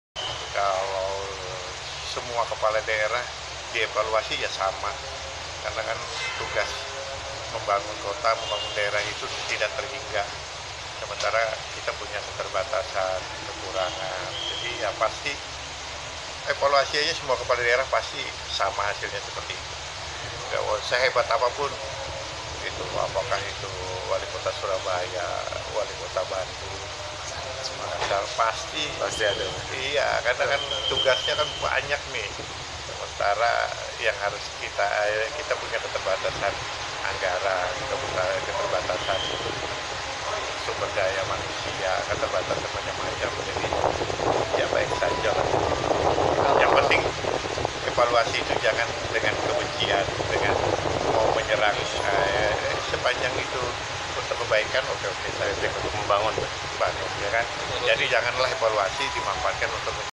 Walikota Balikpapan - mengaku banyak kekurangan selama memimpin
Walikota Balikpapan – Rizal Effendi mengaku masih banyak kekurangan selama memimpin bersama Wakil Walikota Balikpapan – Rahmad Masud.Hal ini terkait sorotan kinerja walikota Balikpapan dan wakil walikota bvalikpapan yang masih banyak kekurangan selama setahun memimpin oleh LSM Stabil kepada awak media, belum lama ini.Walikota Balikpapan,pihaknya tidak akan membenci siapapun yang menyoroti kinerja dirinya bersama wakil walikota.Karena,evaluasi dan sorotan yang diberikan oleh masyarakat maupun organisasi apapun / bertujuan untuk membangun kota ini.Walikota menegaskan, semua kepala daerah di seluruh indonesia tentunya,banyak kekurangan baik walikota Bandung maupun walikota surabaya,sehingga tidak ada kepala daerah yang dalam perjalannya tidak menemukan kendala.Masih banyaknya evaluasi dan kekurangan dalam pendidikan,kesehatan dan pembangunan ini ,dikarenakan faktor keterbatasan anggaran dan sumber daya manusia.Bukan hanya itu, sorotan dan evaluasi kepada kepala daerah harus diterima dengan baik, asalkan evaluasi untuk kepala daerah tidak memiliki tujuan menyerang dan membenci untuk kepentingan lain. Namun demikian, masyarakat perlu tau,bahwa kepemimpinan dirinya dalam membangun kota Balikpapan mendapatkan apresiasi diakui oleh Walikota Makkasar.